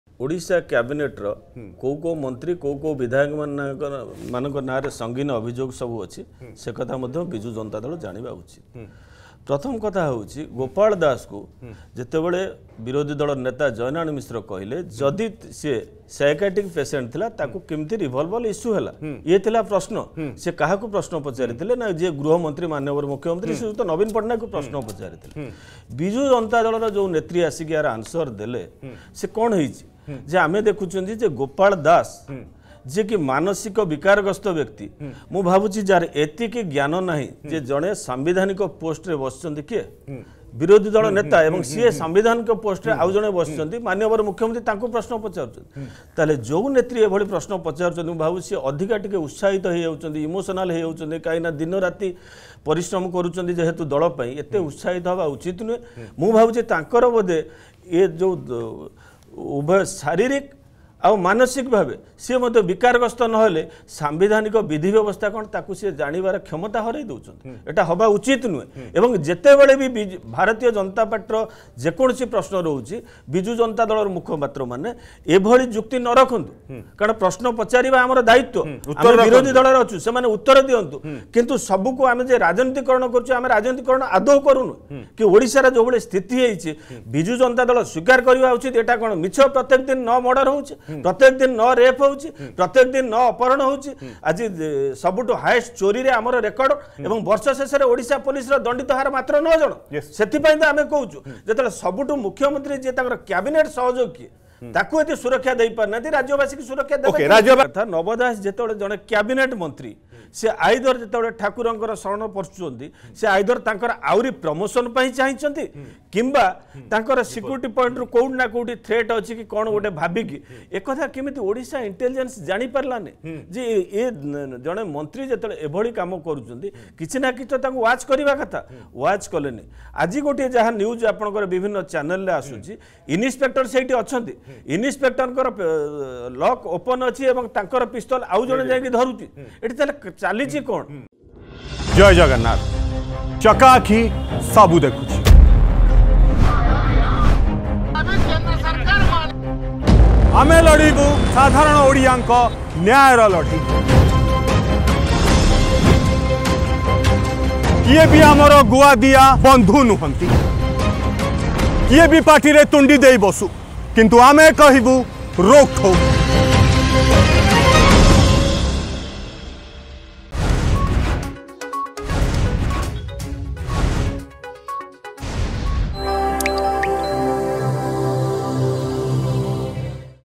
ବିଜେଡି ନେତ୍ରୀ ମାନସିକ ଓ ଶାରିରୀକ ବିକାରଗ୍ରସ୍ତ ହୋଇଯାଇଛନ୍ତି
ବିଜେଡି ନେତ୍ରୀ ମାନସିକ ଓ ଶାରିରୀକ ବିକାରଗ୍ରସ୍ତ ହୋଇଯାଇଛନ୍ତି। ରାଜ୍ୟରେ ସାଧାରଣ ଲୋକଙ୍କ ଜୀବନ ସୁରକ୍ଷିତ ନୁହେଁ: ମନୋଜ ମହାପାତ୍ର, ବିଜେପି ମୁଖପାତ୍ର...।